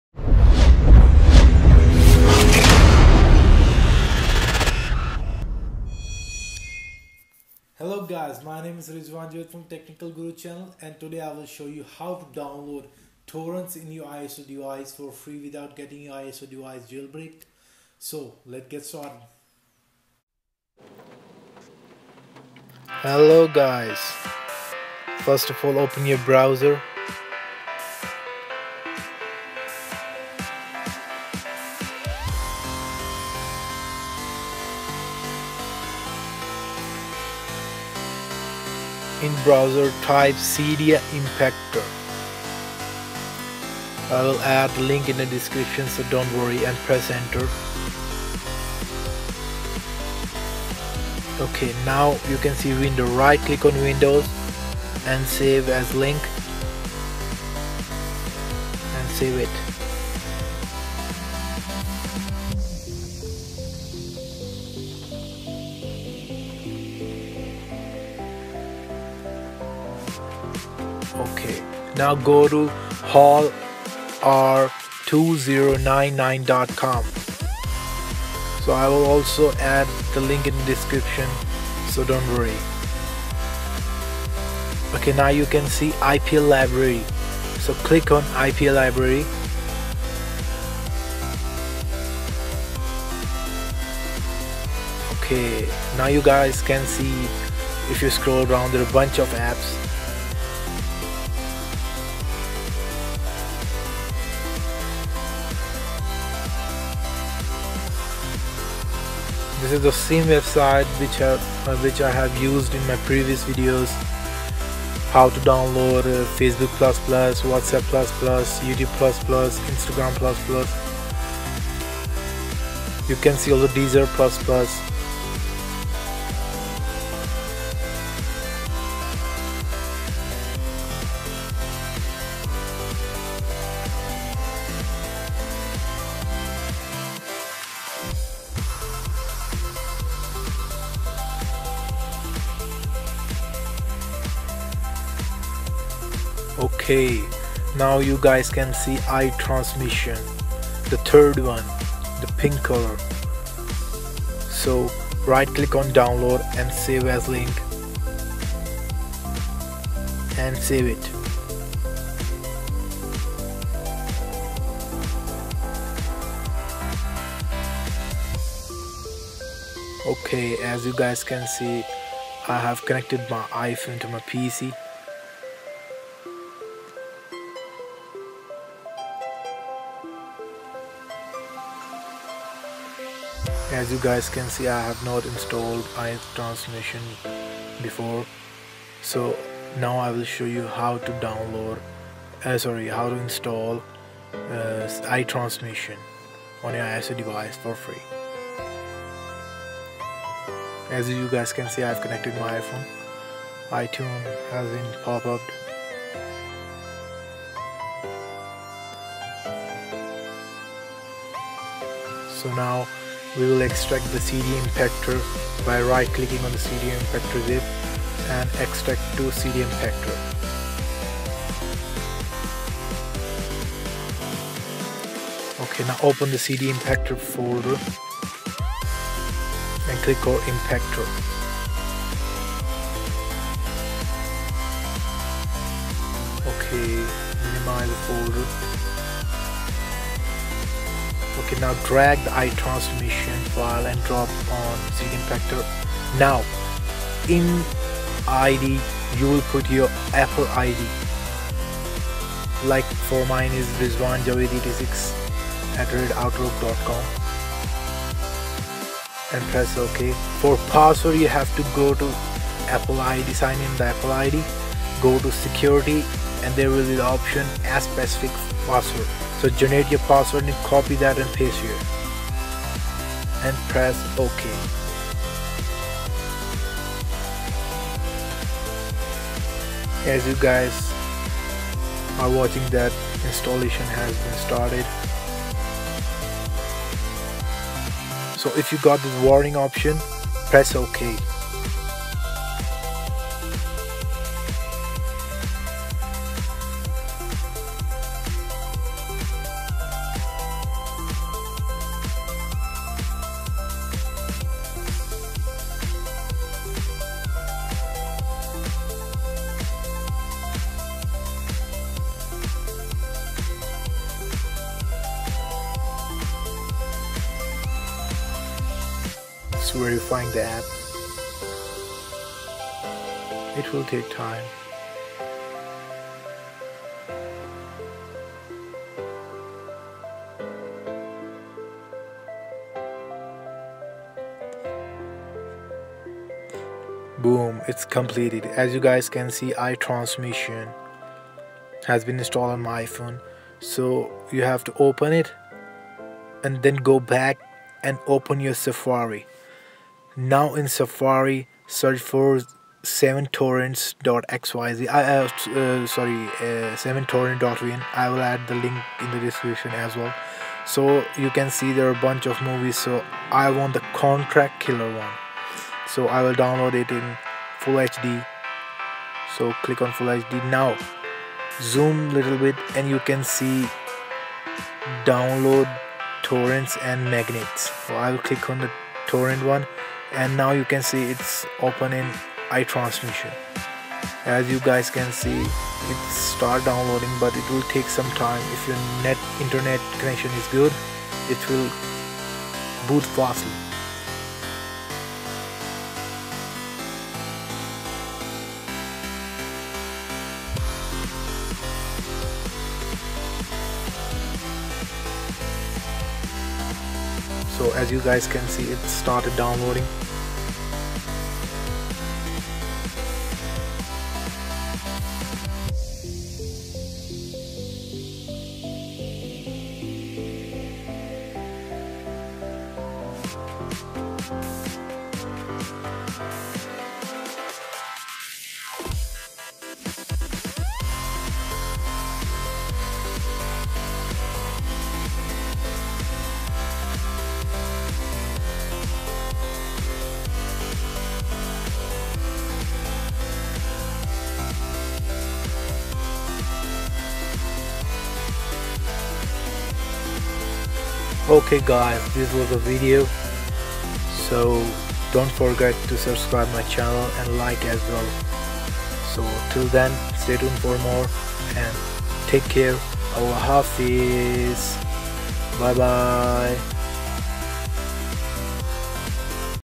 How To download torrents for free (NO JAILBREAK) iPhone, iPad, iPod
in this video i will show you guys How To download torrents  for free (NO JAILBREAK) iPhone, iPad, iPod